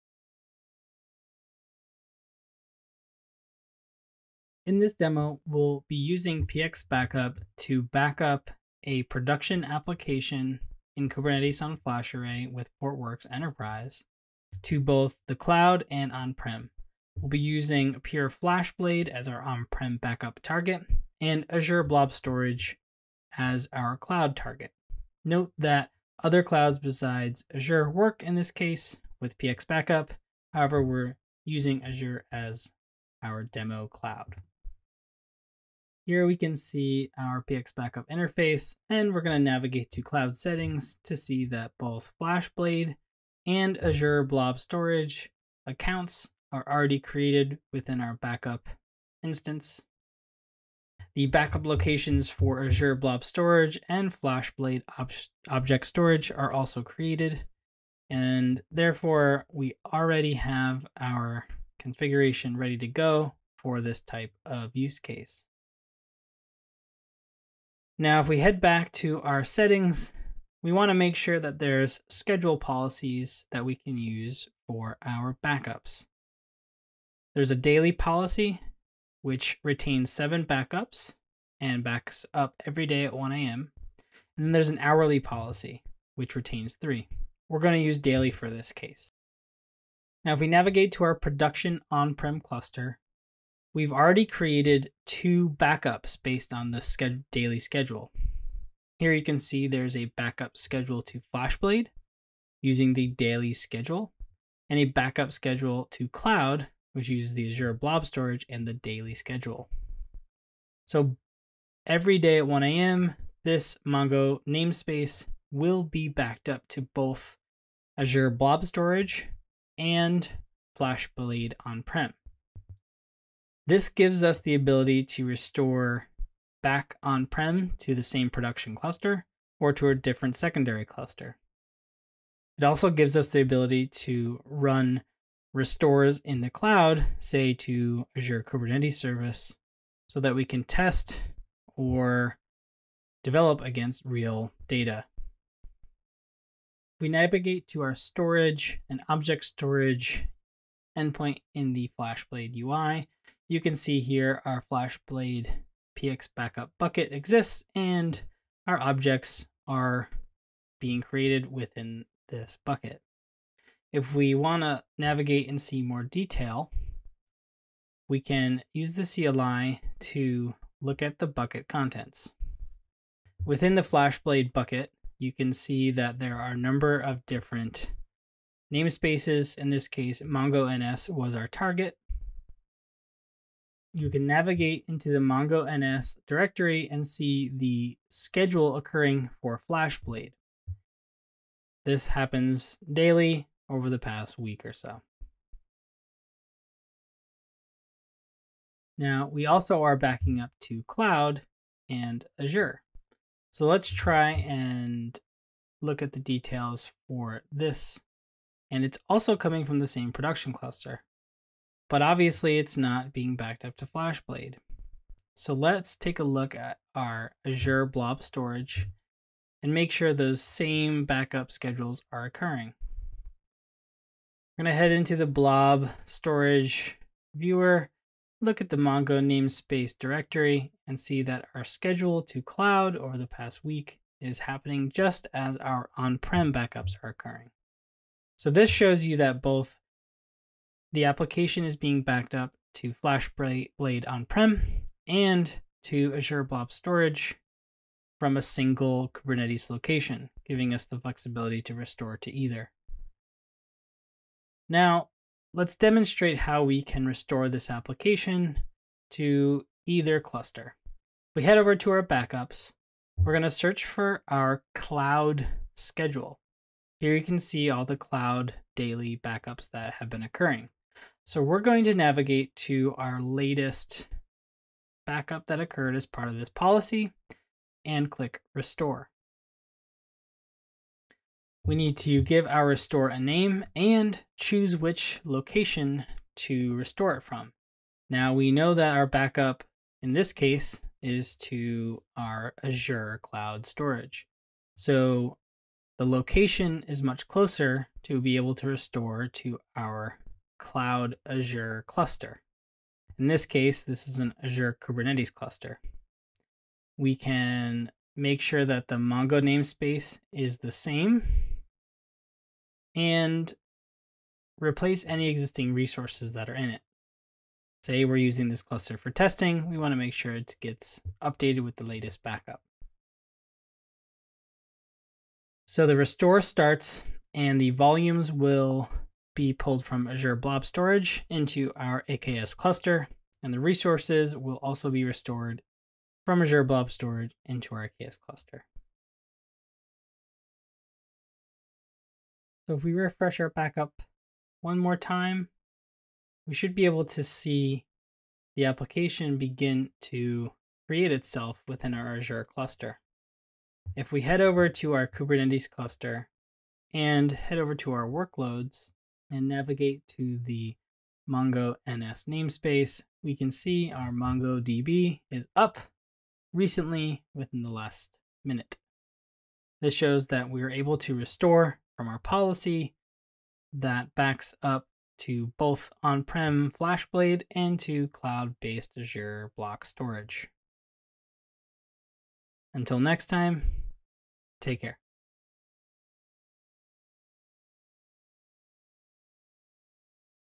Cloud Backups for Additional Protection
PX-Backup enables you to protect your entire data center and store applications in the cloud for test/dev. PX-Backup can send a copy of your Kubernetes applications to both a FlashBlade on-premises as well as an AWS S3-compatible object store in the cloud. You get both fast recovery of on-prem production applications and a backup copy in the cloud should you need it.